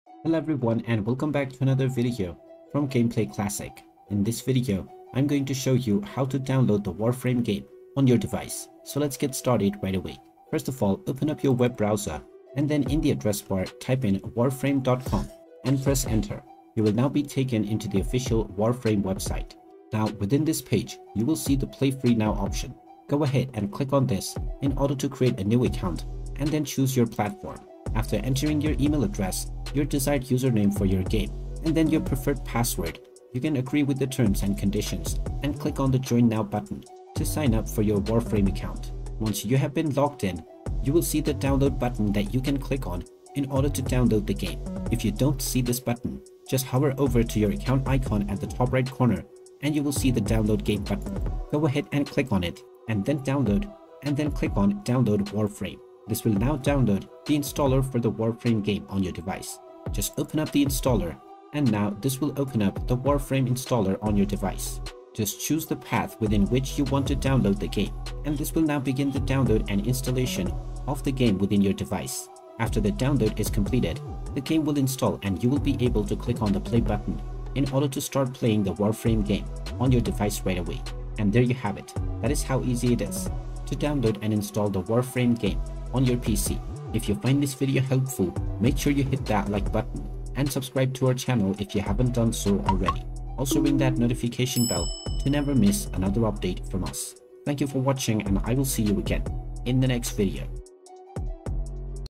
How to Download Warframe Game on Your PC? Install Warframe Game on Your Laptop (2024)
In this video, I'll guide you through the process of downloading and installing Warframe on your PC. Warframe is an action-packed, free-to-play online game that lets you explore a vast universe while battling enemies in intense co-op missions. Whether you're new to the game or a returning player, this step-by-step tutorial will show you how to quickly get the game up and running on your computer so you can jump into the action!

Thank you, viewers, for your continuous support! We advise against sharing personal credentials to keep the internet safe and build a better gaming community.